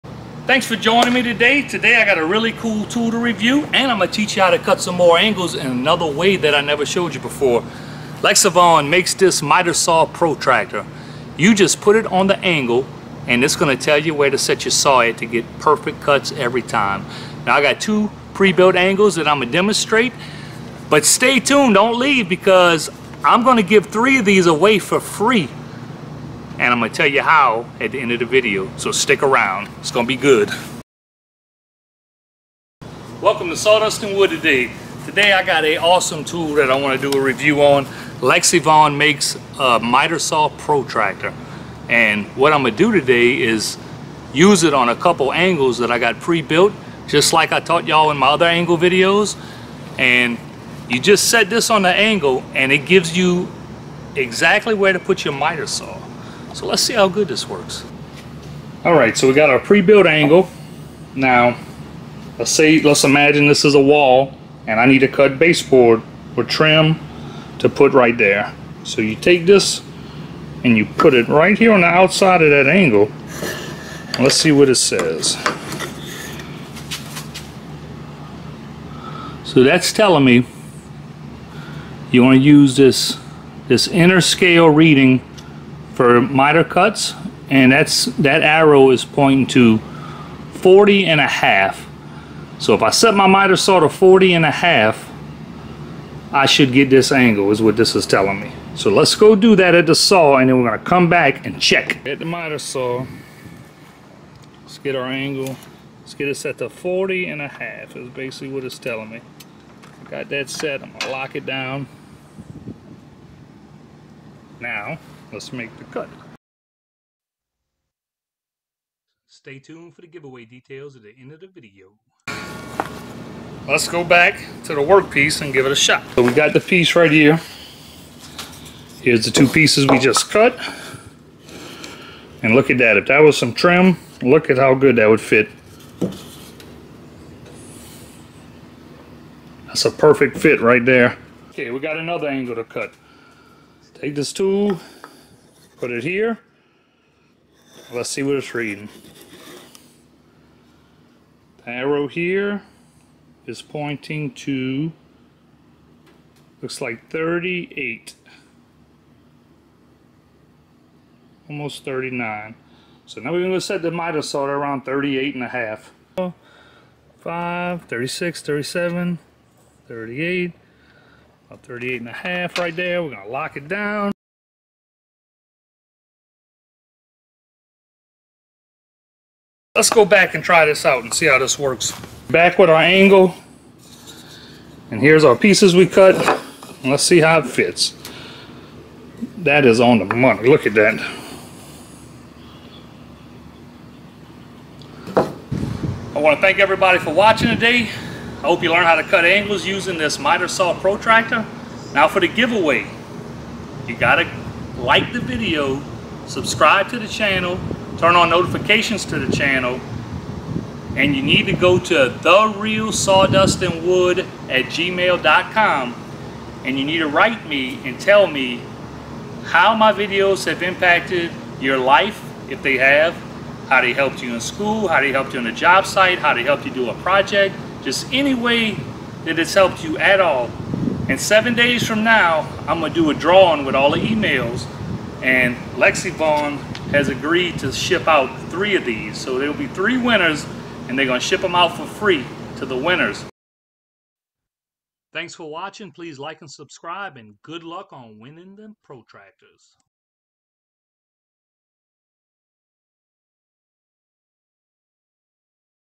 How to cut any angle using Lexivon's miter saw protractor!!
This video I teach how to find angles for trim and set your miter saw correctly. I use Lexivon's miter saw protractor.